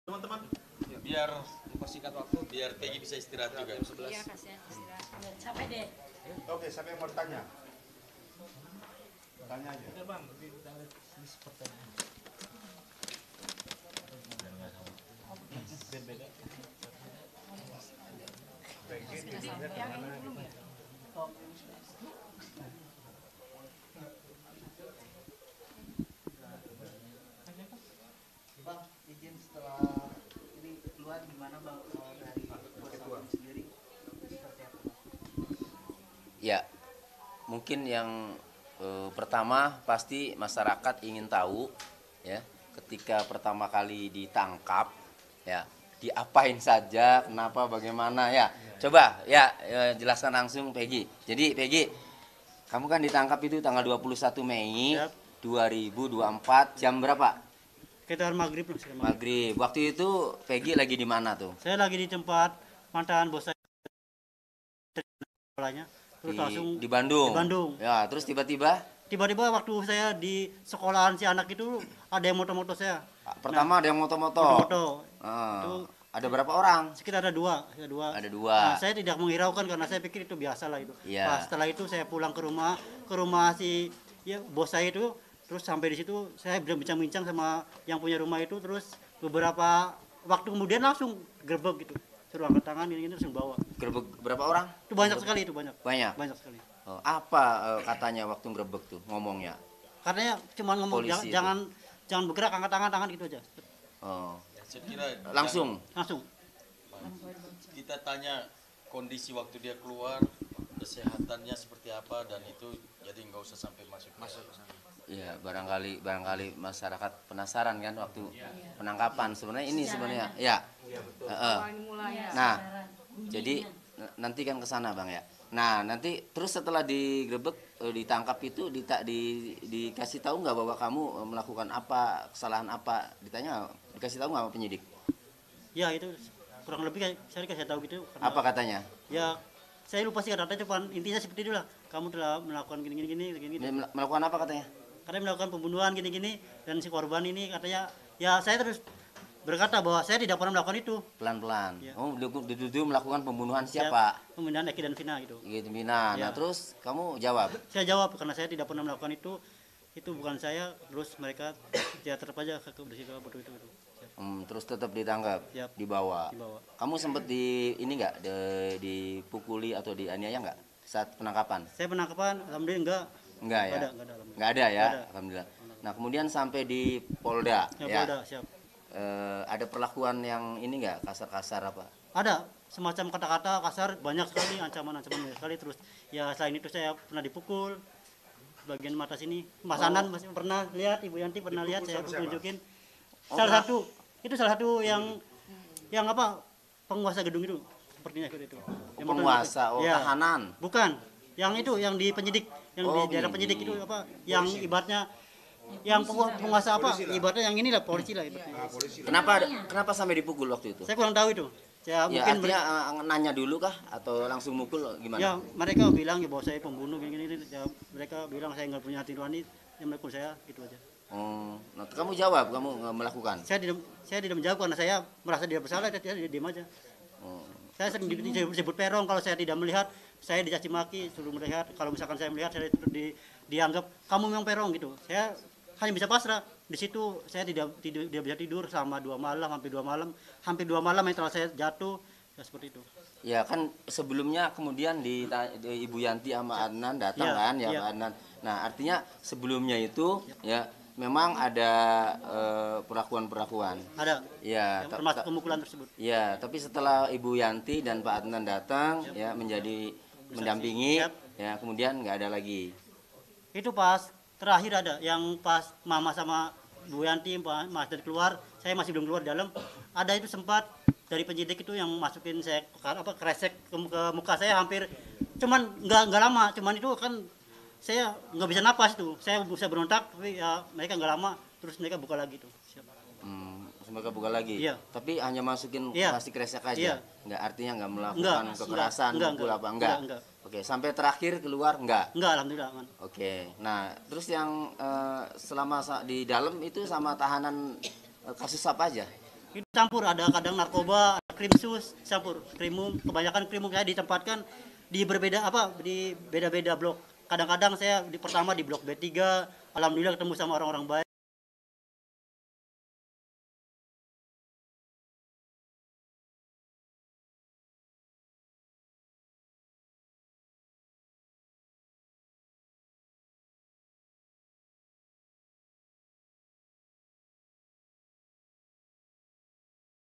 Blak-blakan! Pegi Setiawan Cerita Awal Penangkapan oleh Polda Jabar: Saya Pernah Dipukul Bagian Mata
KOMPAS.TV - Blak-blakan! Pegi Setiawan Cerita Awal Penangkapan oleh Polda Jabar: Saya Pernah Dipukul Bagian Mata